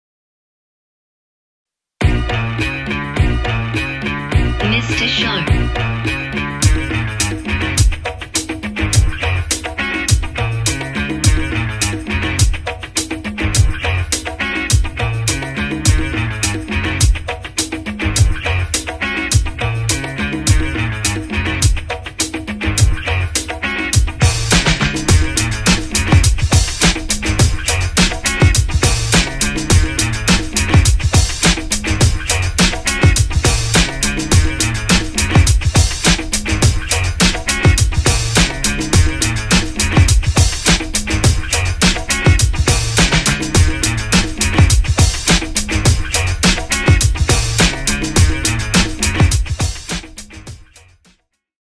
Lafayette 2018 AUTUMN/WINTER COLLECTION
2018/07/28(sat) Delivery Start.
ー　 Starring : Alejandro “AT” Tavarez /K!D/CaveMitich/Lashy/Simone Embrack
ー　 Photographer : Eli Salcedo
ー　 Movie : Eli Salcedo
ー　 Music : MISTA SHAR @mista_shar
ー　 Direction & Design : Lafayette @Lafayette_NYC